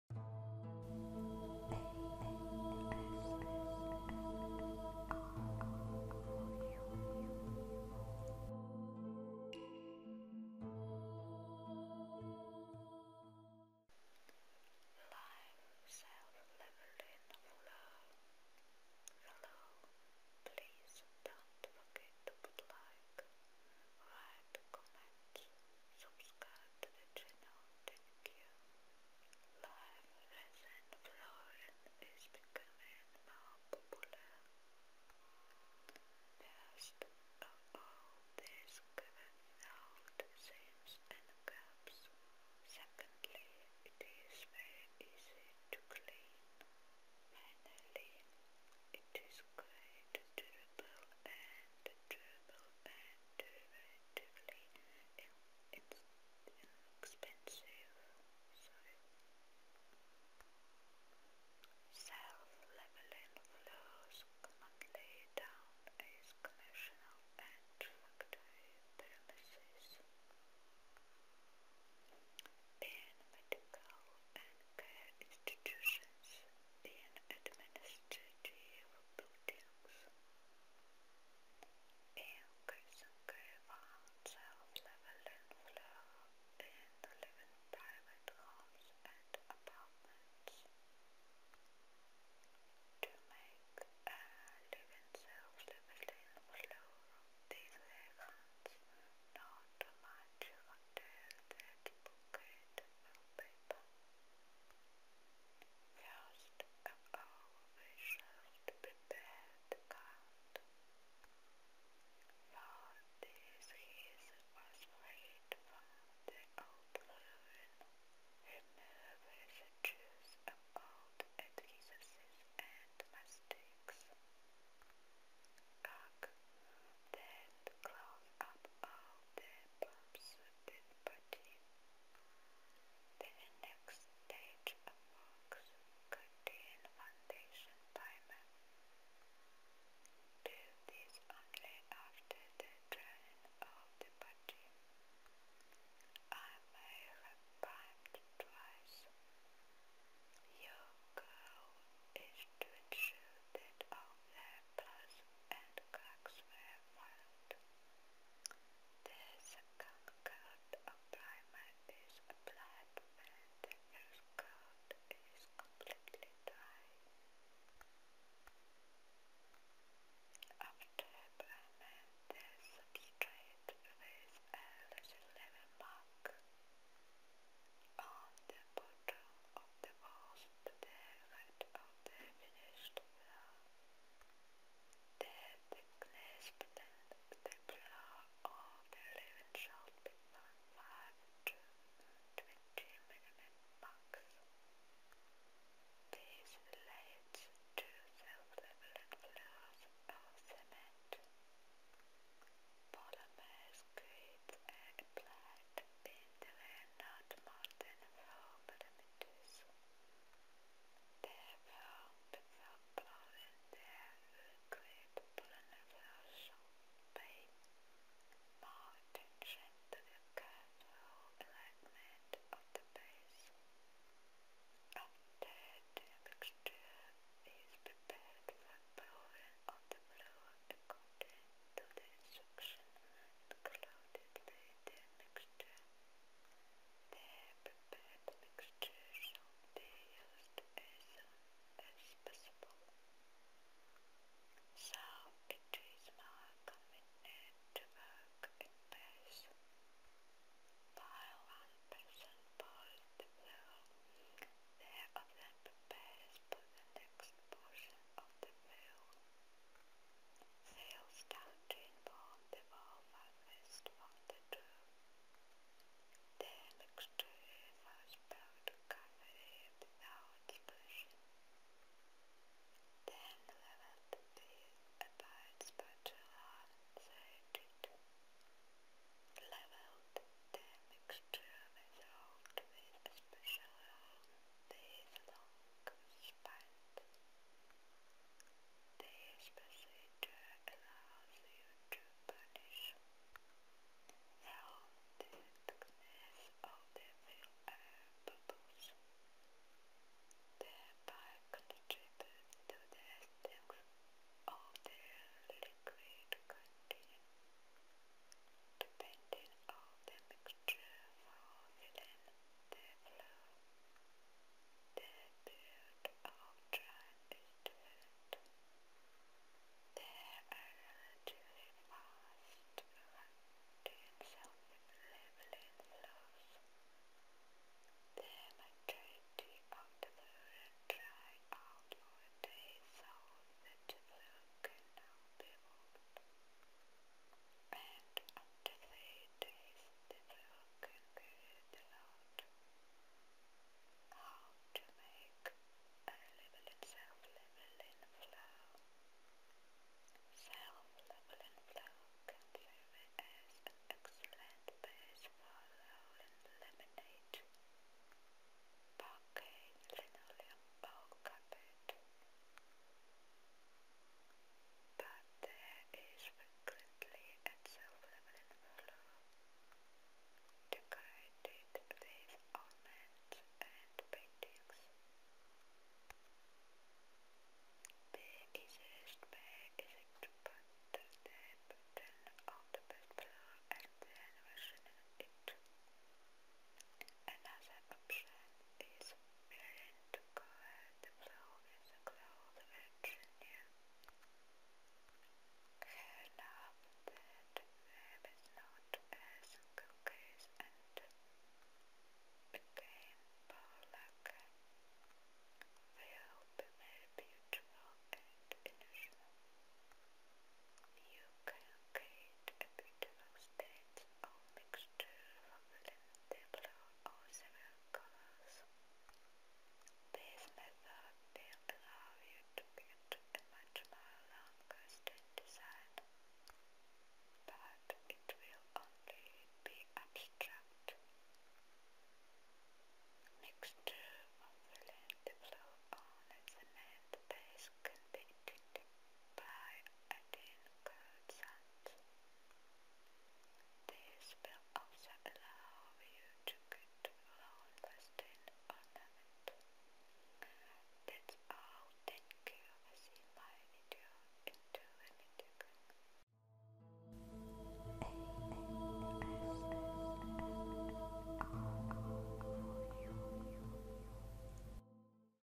ASMR Live self leveling floor
Live self-leveling floor.
Live resin flooring is becoming more popular. First of all, this cover without seams and gaps. Secondly, it is very easy to clean. Finally, it is quite durable and durable and relatively inexpensive...
00:00-01:44 - Live self-leveling floor
01:44-02:11 - First of all, we should prepare the ground
02:11-05:42 - The next stage of works
05:42-07:45 - How to make a living self-leveling floor
For the development of the channel : Webmoney Z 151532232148